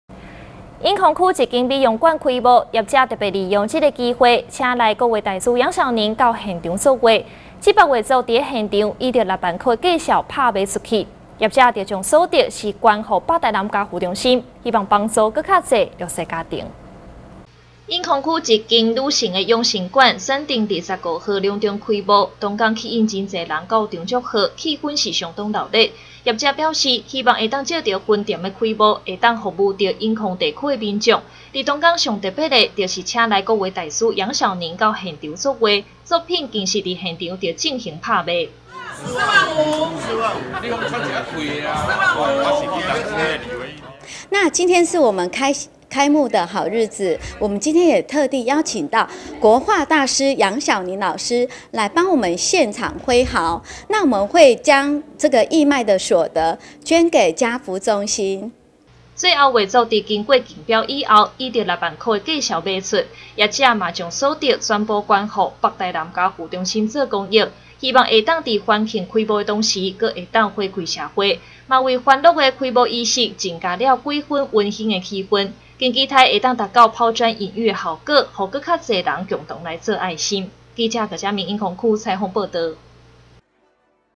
1070917新永安新聞-開幕做公益 美容業者義賣名畫助家扶
永康區一家美容館開幕 業者特別利用這個機會 請來國畫大師楊小寧到現場作畫 這幅畫作在現場以6萬元的價格拍賣出去 業者將所得捐給北臺南家扶中心希望幫助更多弱勢家庭